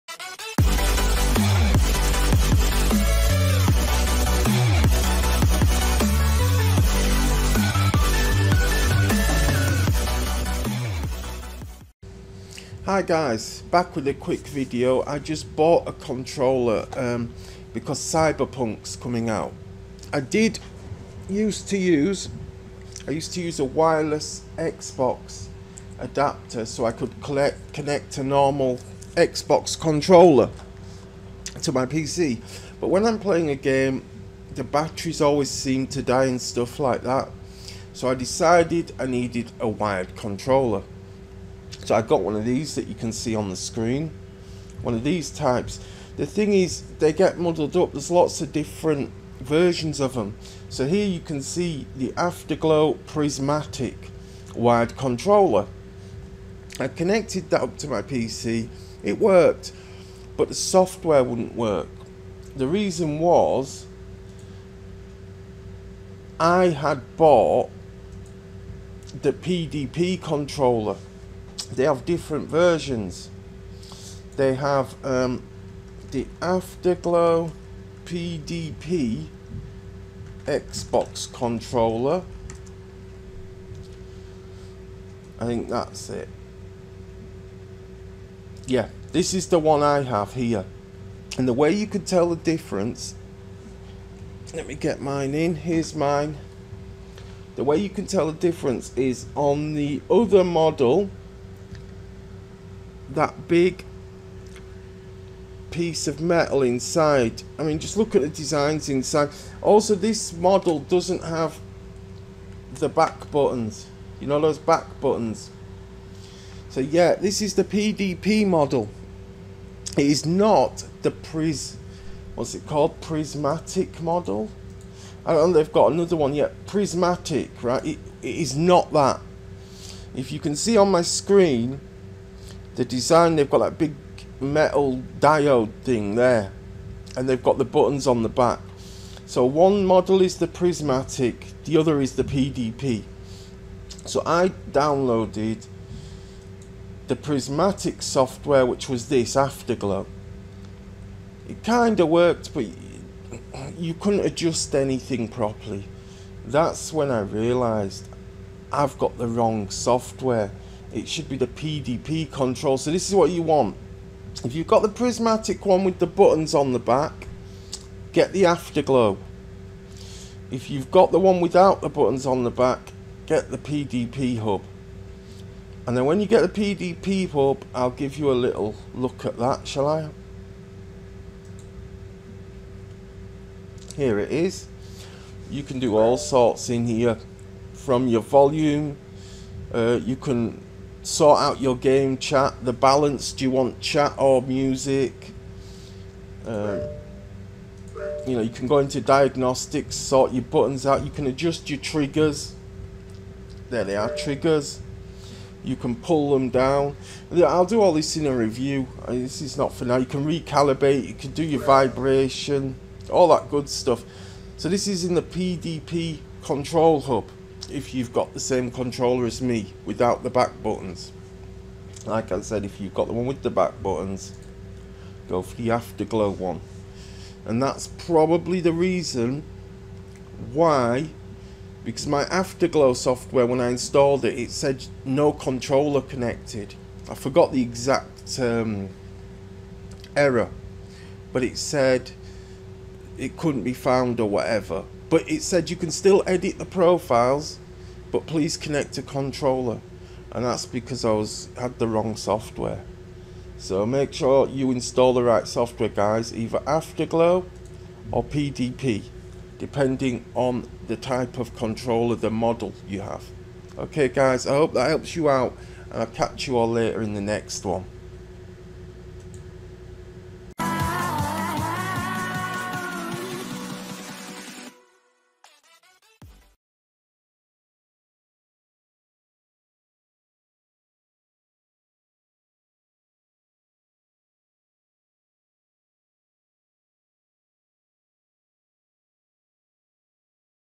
Afterglow Prismatic Controller not connected PC FIX
Afterglow Controller not connected PC FIX

So my afterglow controller software said there was no controller connected, this was my fix.  The actual error I got was

"an afterglow prismatic controller is currently not connected"

this was infact correct as I had an Afterglow pdp xbox and pc controller and so needed the PDP software not the afterglow one!